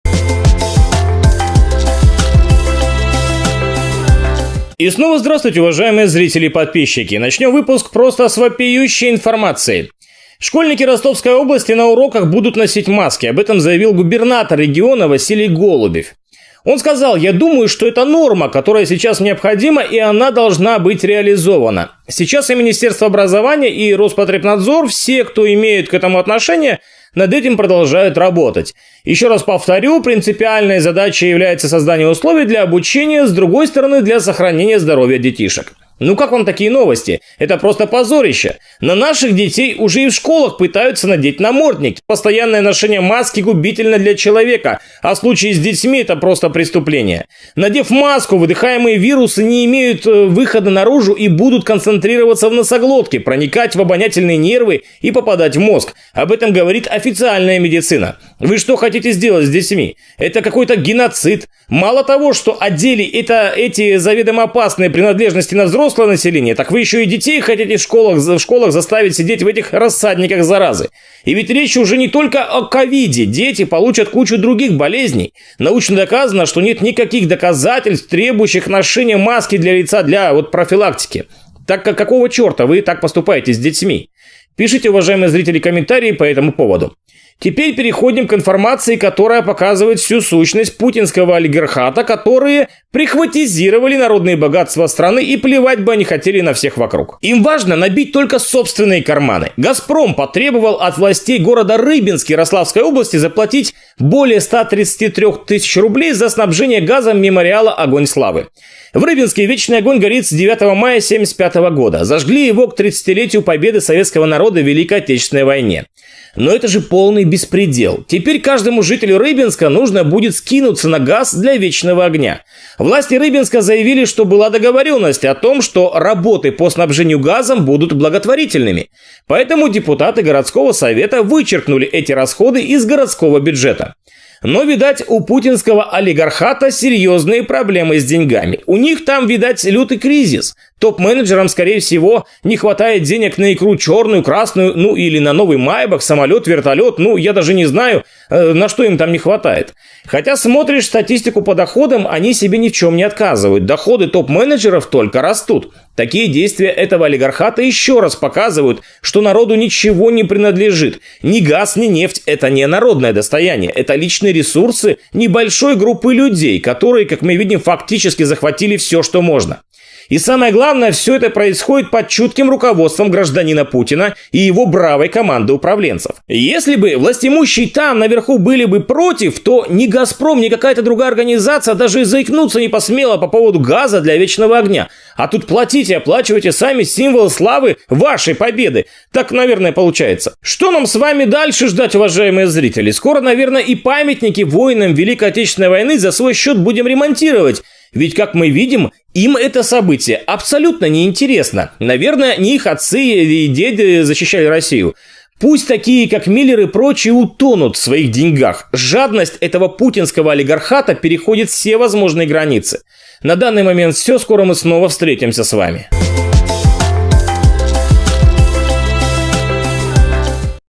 Миллер требует деньги за газ для вечного огня!
Появилась информация, что некоторые регионы введут для школьников обязательное ношение масок. Данный факт просто противоречит здравому смыслу. Это просто нонсенс. Чего добивается Путин? Явно данное требование не прибавит здоровья школьникам. Также мы обсудим требования Миллера и Газпрома к населению. Вопиющий случай произошёл в городе Рыбинске.Жадность друзей Путина переходит все границы. Подробности в данном выпуске.

Спасибо за поддержку нашего канала!